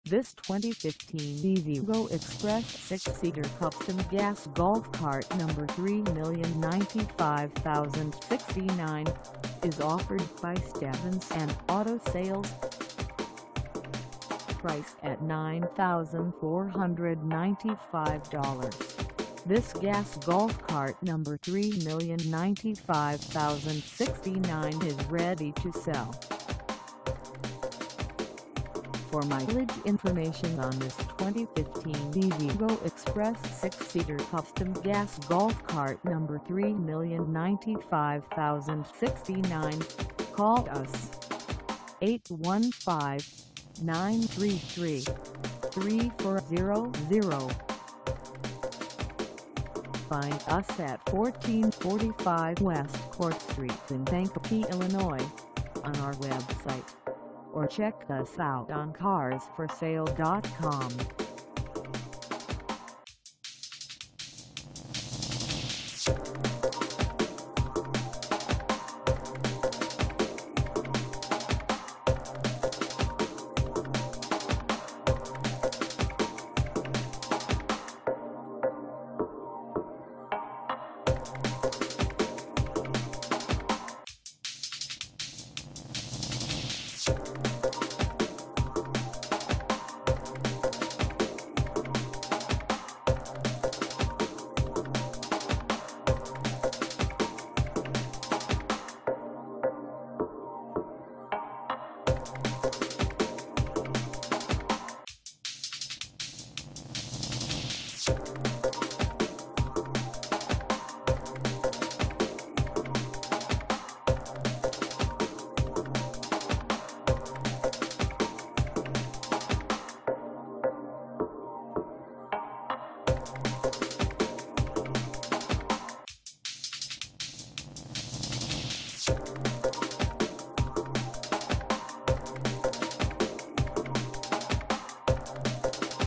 2015 EZ GO EXPRESS 6 SEATER CUSTOM GAS GOLF CART #3095069 f
This 2015 EZ GO EXPRESS 6 SEATER CUSTOM GAS GOLF CART #3095069  is for sale in Kankakee, IL 60901 at Stevenson Auto Sales.
Engine:
Transmission:
Color: Maroon
Mileage: 0
Contact Stevenson Auto Sales 1445 W Court St Kankakee, IL 60901 to test drive this 2015 EZ GO EXPRESS 6 SEATER CUSTOM GAS GOLF CART #3095069  today.